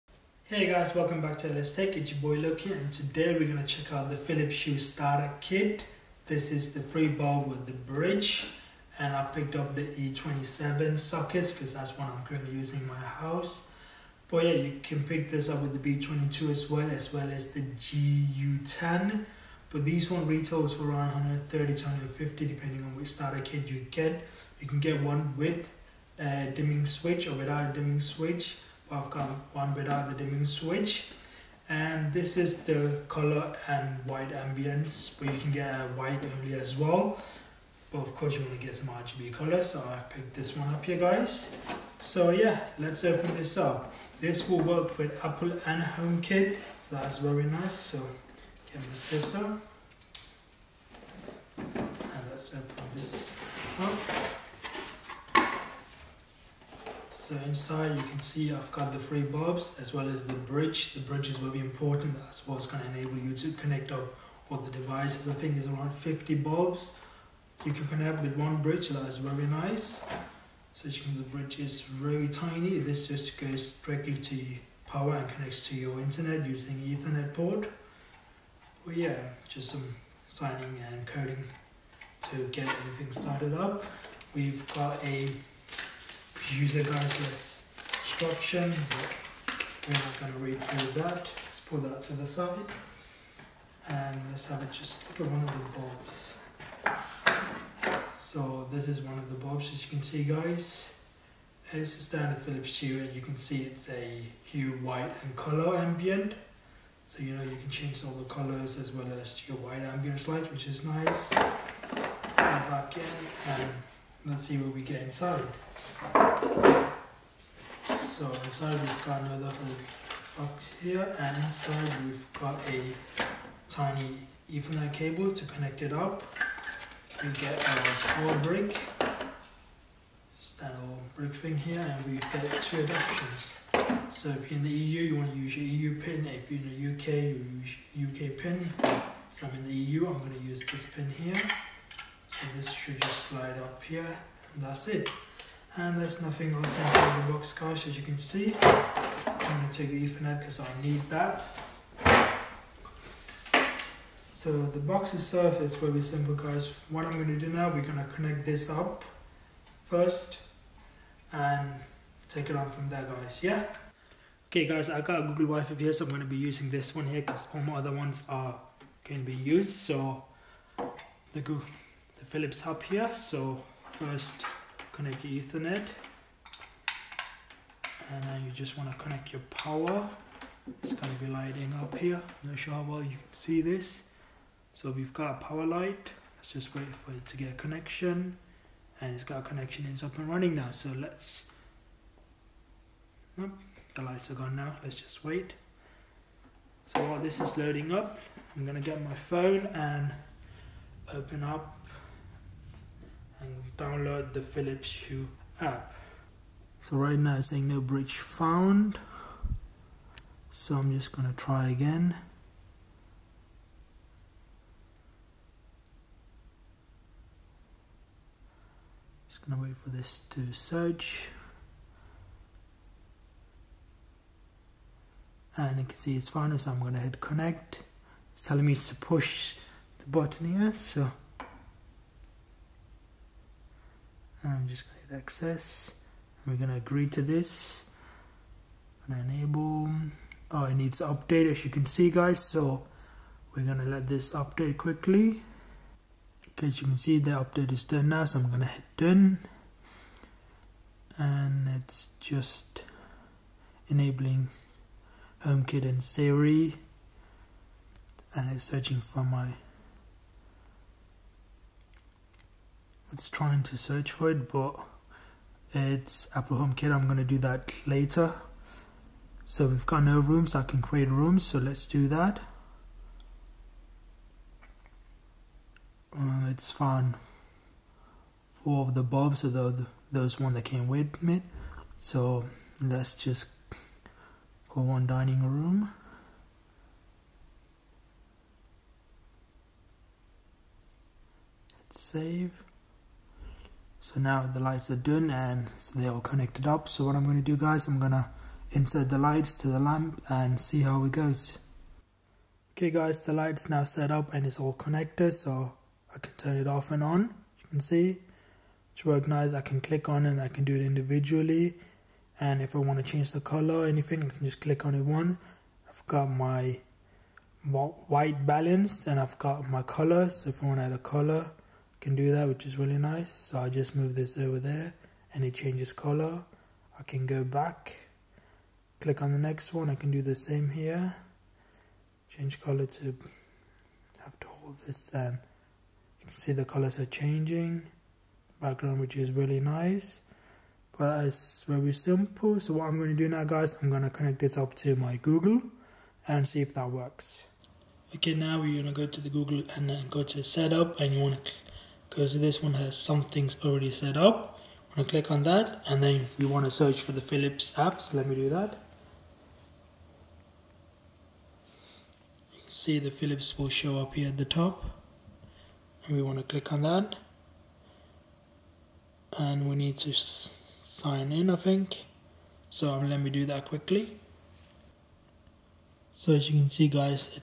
Philips Hue Starter Kit 3xE27
Hey guys, welcome back to LS Tech.
Today we are checking out the Philips Hue Starter Kit 3xE27

★ Amazon UK

🎥 Film Gear Amazon UK
Sony A7Rii
Zeiss Batis 25/F2
Sony A6300
Sony 18-105/F4
Sony 50/F1.8
Sony Tripod